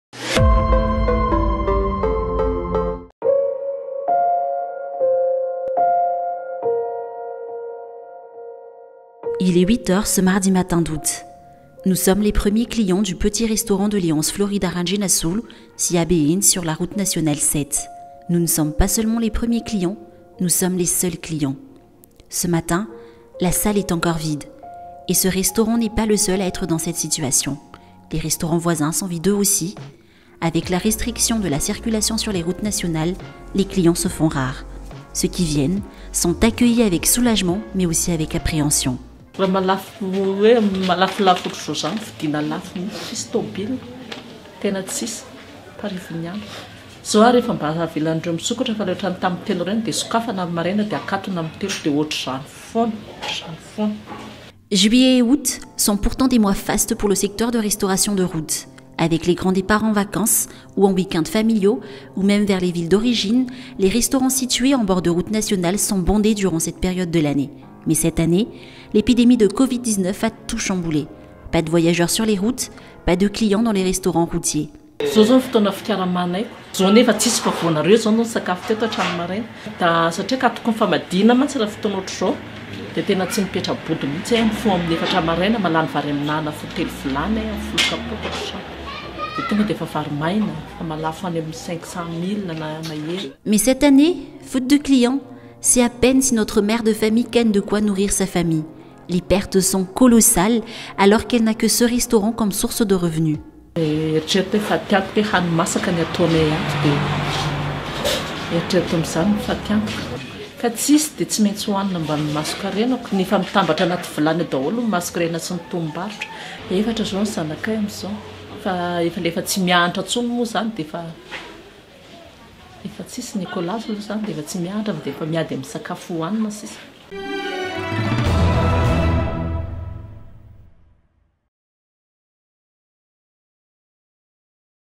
CIRCULATION – Le goût amer du confinement pour les restaurateurs routiers
La restauration de route est l'un des secteurs les plus touchés par la crise sanitaire que le pays traverse. Avec la réglementation de la circulation sur les routes nationales, les clients sont aux abonnés absents. Chez Léonce Florida Randrianasolo, restauratrice sur la Route Nationale 7, à Behenjy notamment, le moindre voyageur est accueilli avec soulagement, mais avec amertume aussi : le prix de revient d'un plat, quand on n'en sert qu'en petite quantité, lui revient cher. Très cher.